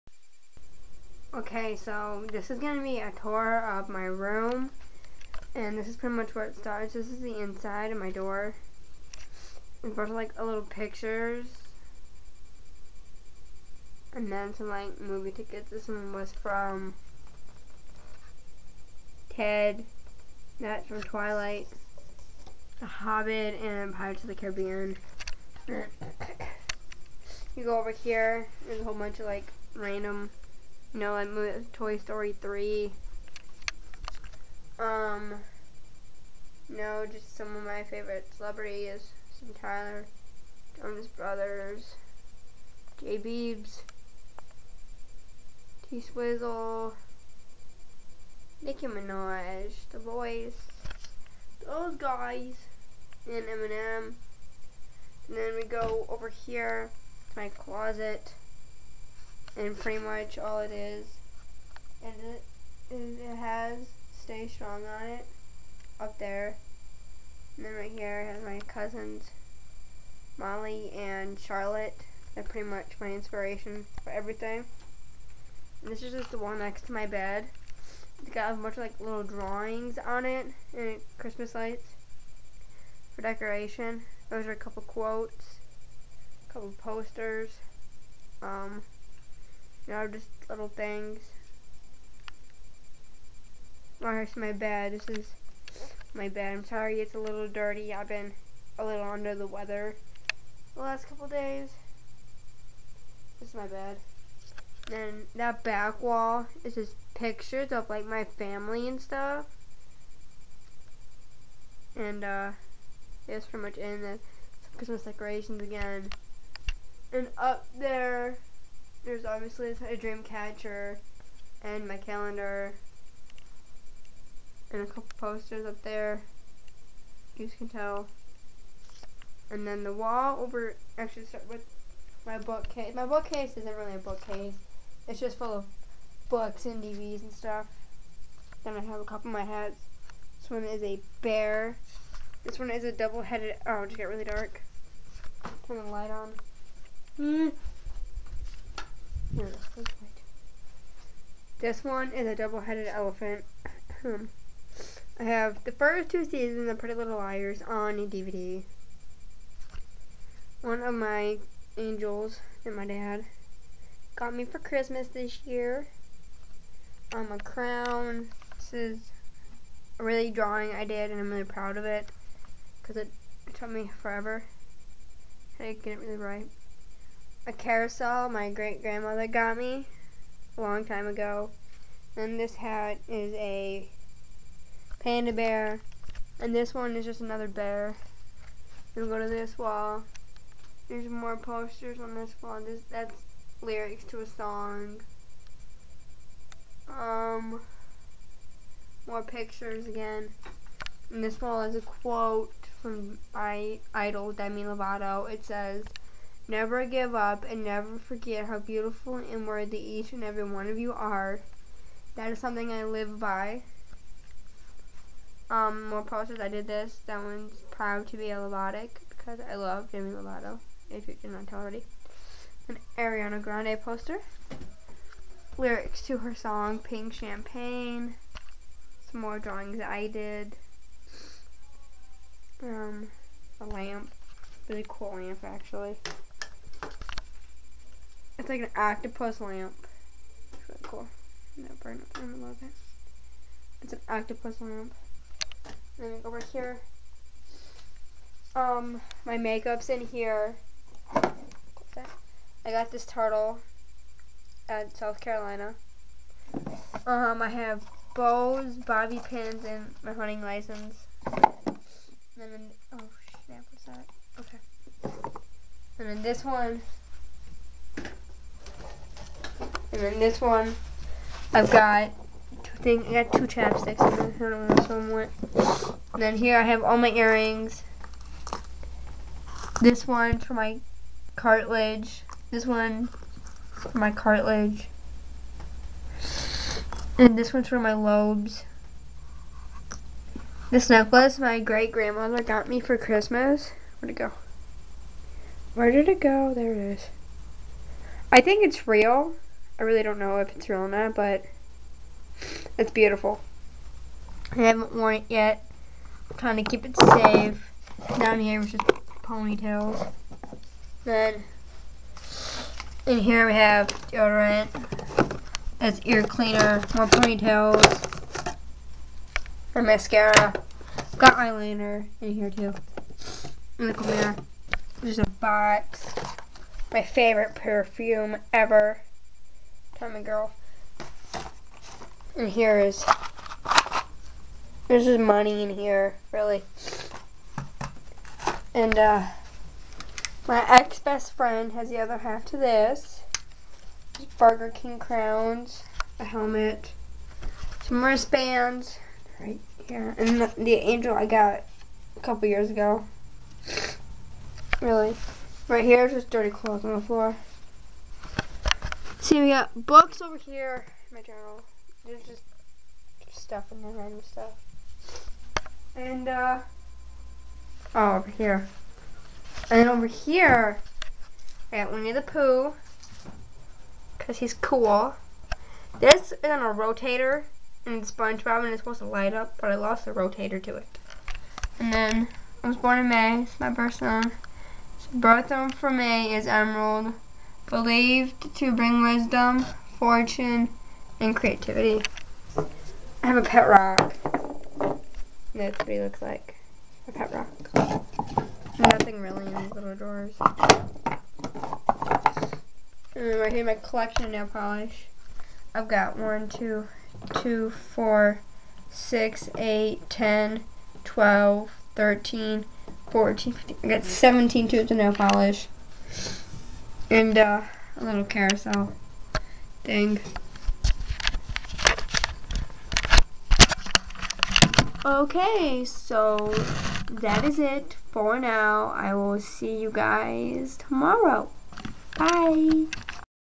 Room Tour! (Day 1 2-19-13)
Sorry i sound really froggey, i'm still recovering from being sick and decided i should put up a video. Again, we're in the process of moving so these may or may not be everyday, i will try tho. Please stay with me! xo